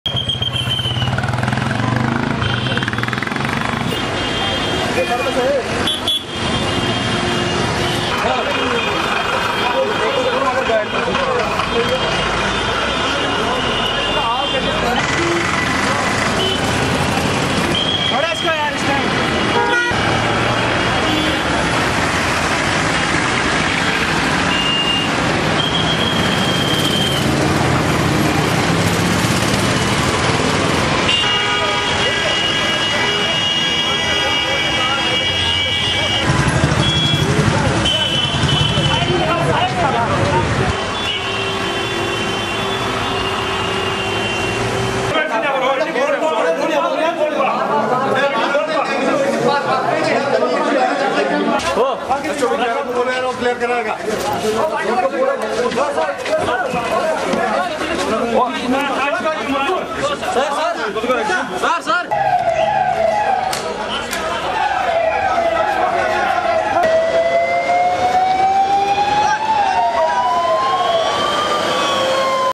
ANANTNAG.. UNIDENTIFIED GUMEN FIRE A CONSTABLE OF J&K POLICE
ADMITTED IN HOSPITAL.